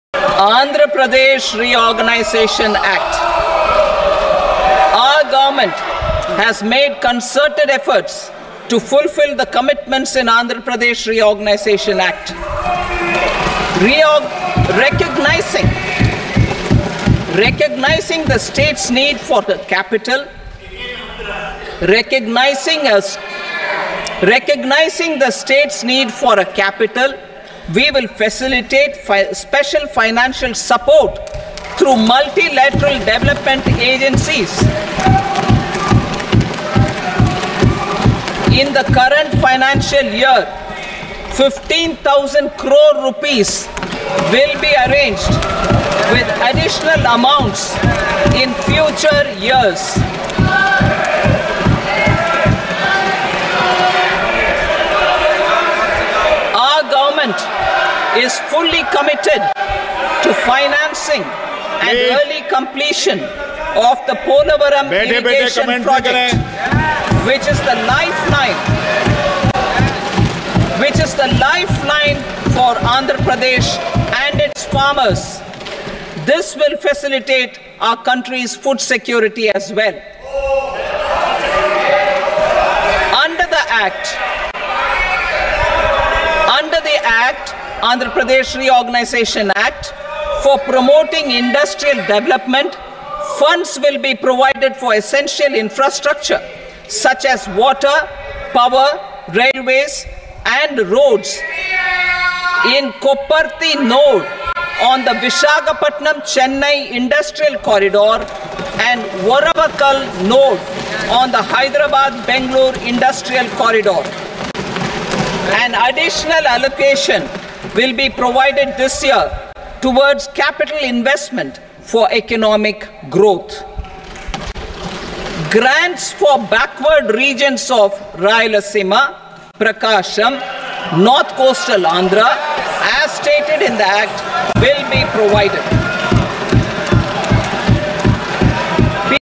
అమరావతికి 15 వేల కోట్లు..|Finance Minister Nirmala Sitharaman Good News to AP Devolopment AP Capital
Watch► అమరావతికి 15 వేల కోట్లు..|Finance Minister Nirmala Sitharaman Good News to AP Devolopment AP Capital